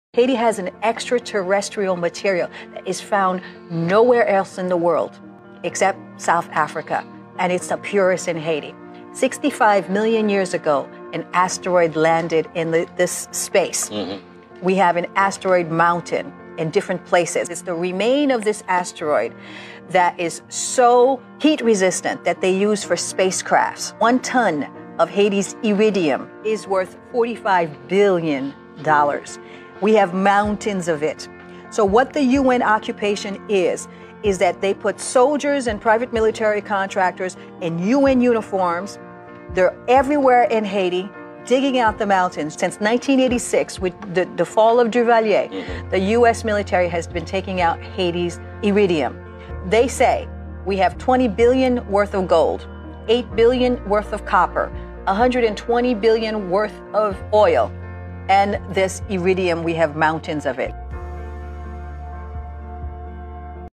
Why the Collapse of Haiti ...? Must watch. !
😳 “One ton of iridium is worth $45 billion and Haiti has mountains”

⚠️Is this the real reason behind the collapse of Haiti???